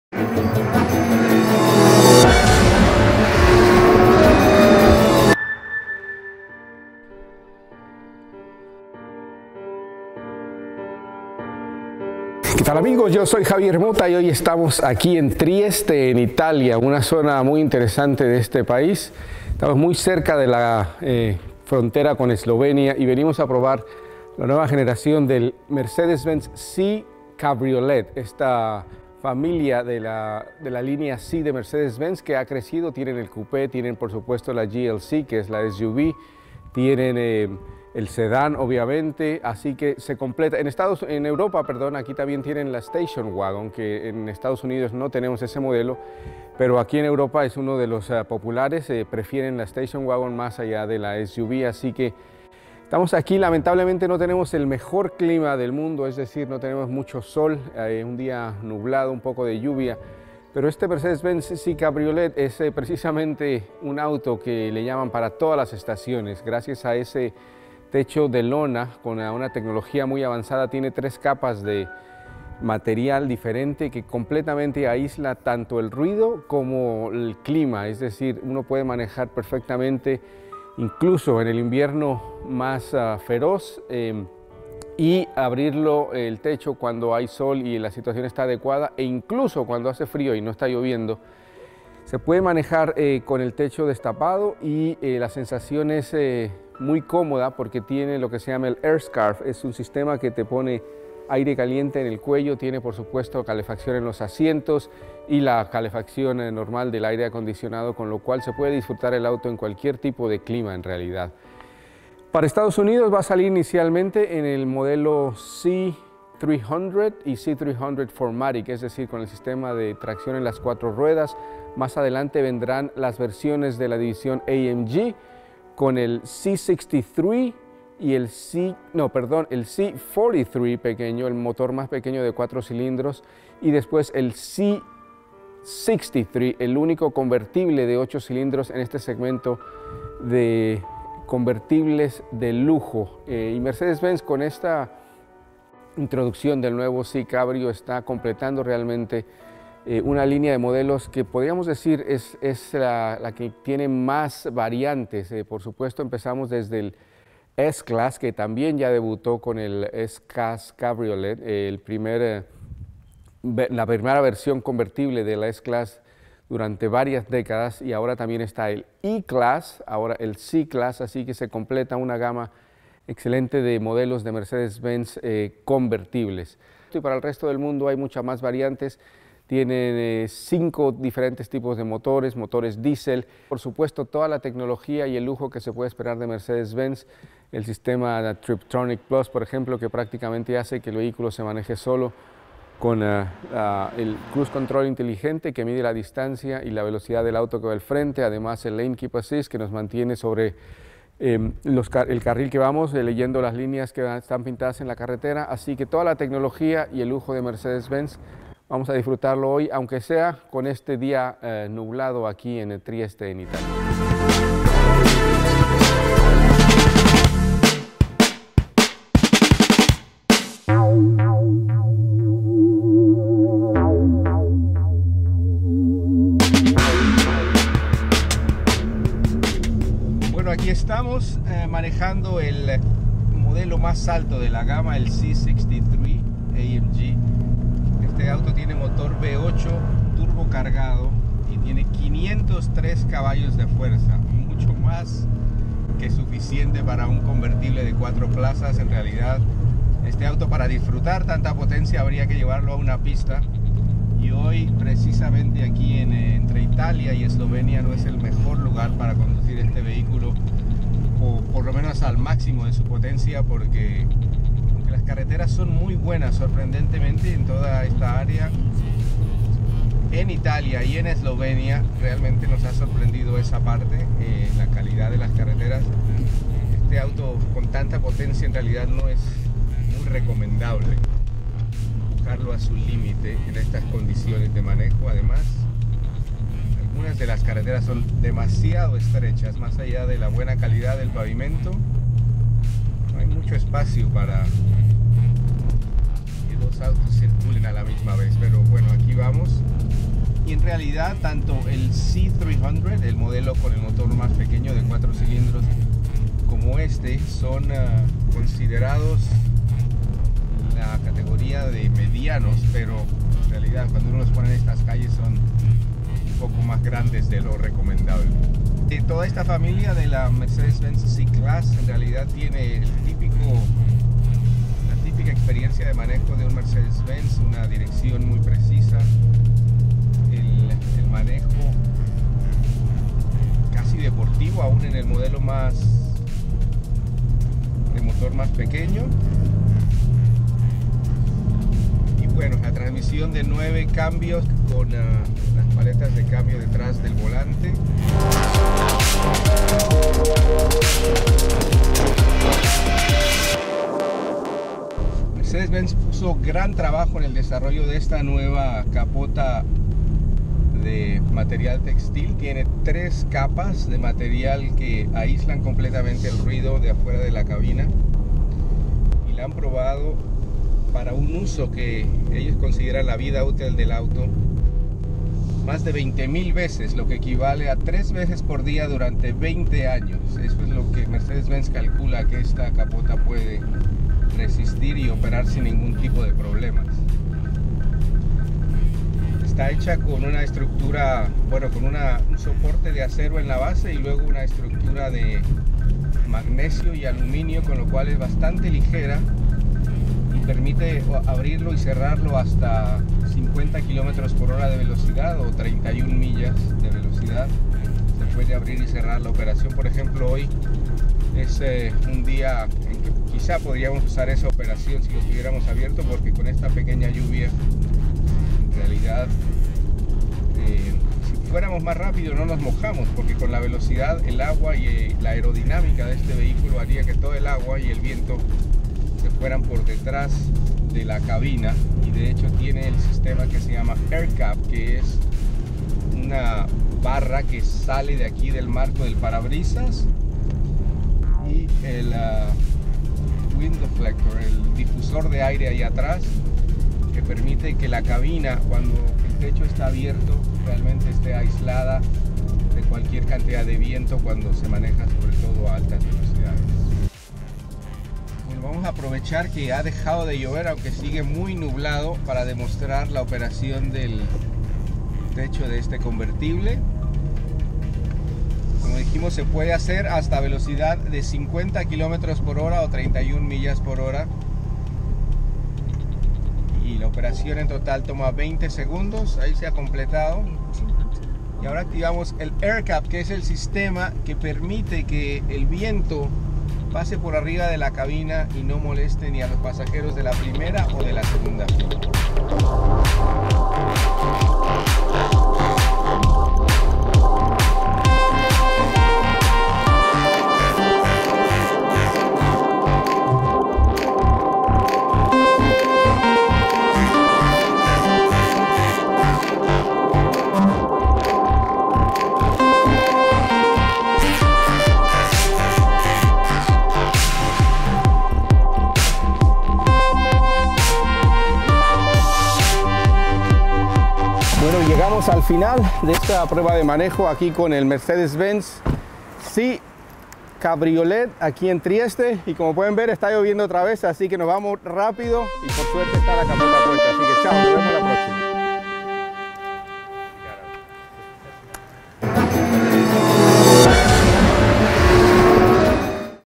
Test Drive Mercedes-Benz C Class Cabriolet 2017 en Trieste, Italia - Español
Viajamos a Tireste, Italia para el Test Drive Mercedes-Benz C Class Cabriolet 2017 y comprobamos que una vez más la marca alemana está adelante de la competencia en diseño, experiencia de manejo y sobre todo en tecnología de seguridad, comunicación y entretenimiento.